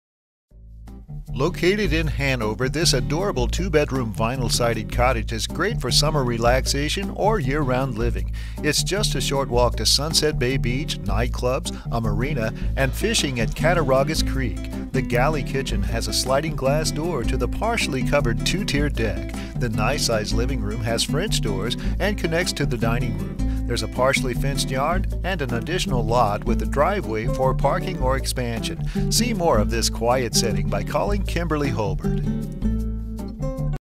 Home for sale in Hanover, NY | $79,900
SUNSET BAY COTTAGE!!! Short walk to Sunset Bay Beach, night clubs, marina &amp; fish at Cattaraugus Creek. Adorable 2 bedroom vinyl sided cottage for summer relaxation or year round living. Nice size living room w/french doors, dining room. Galley kitchen w/sliding glass door to partially covered 2 tier deck. 6x6 laundry room formally used as bedroom. Updated bath. Front porch, storage under rear deck, fully insulated attic &amp; floors 2008, partially fenced yard. Additional lot SBL# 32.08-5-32 w/driveway for parking. Expand or build a garage. 3 marinas for boat launch